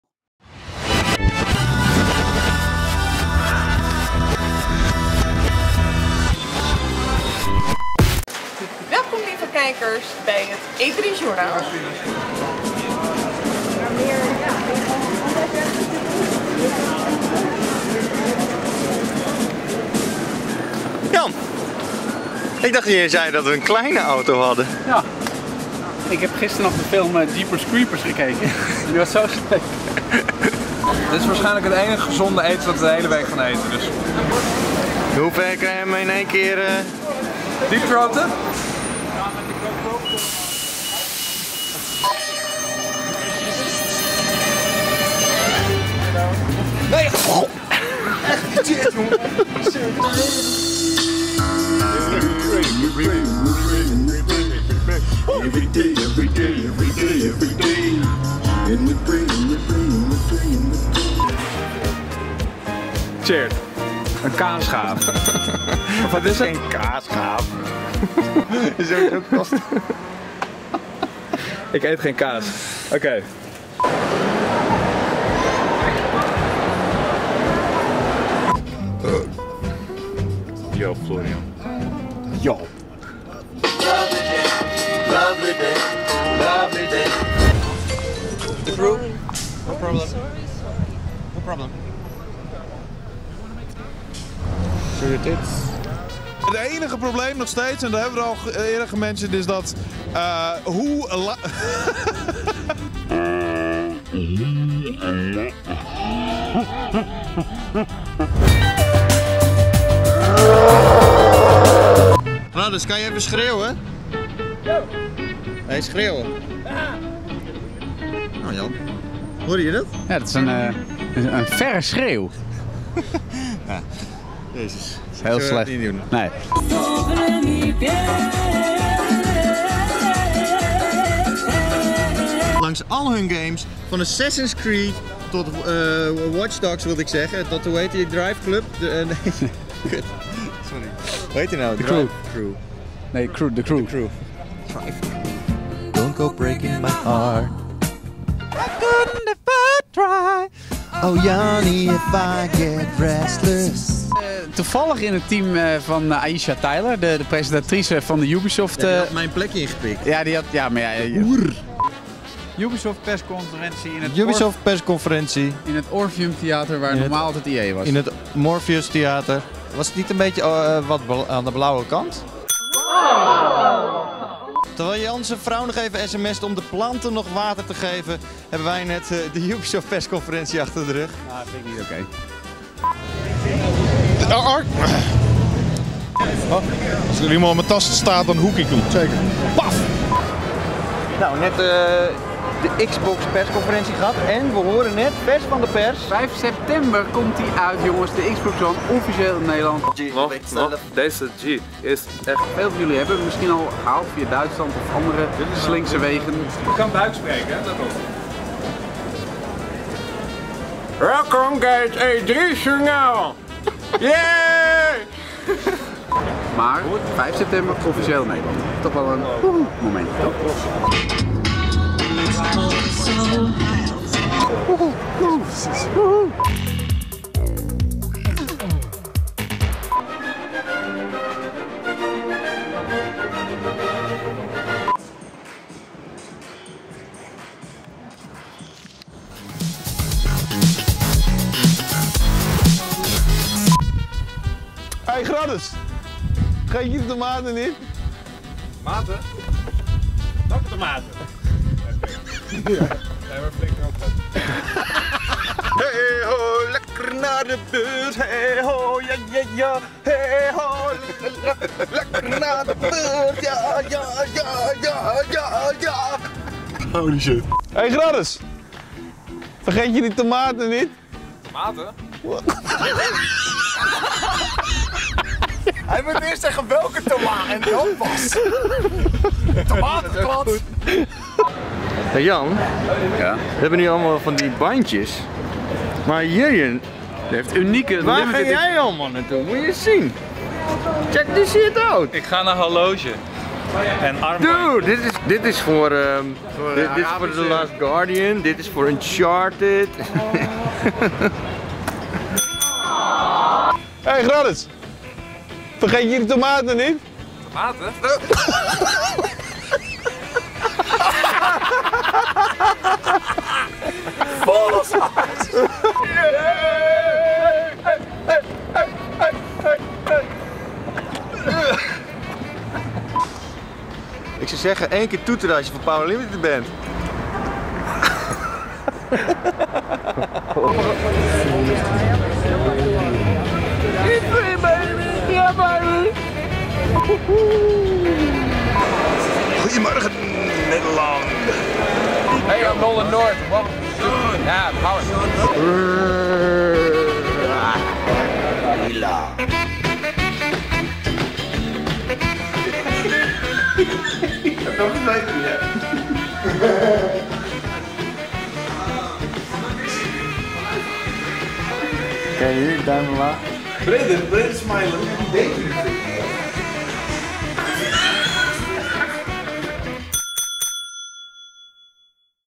E3 Journaal 2014: Bloopers en Overige Banale Bijdrages
Omdat er bij opnames voor óf achter de camera altijd wel iets misgaat, hadden we na de E3 natuurlijk een stapel met outtakes van heb ik jou daar. We hebben de leukste, grappigste, domste en alles wat daar tussenin zit voor je op een rijtje gezet!

Wil je meer Power Unlimited? Check dan zeker